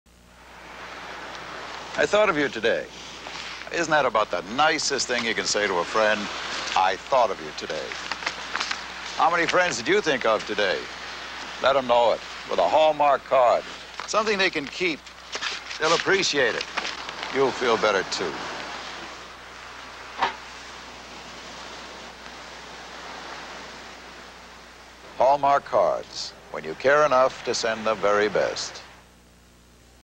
Hallmark Card 1978 Commercial
1978 commercial for Hallmark greeting cards. Aired January 3, 1979.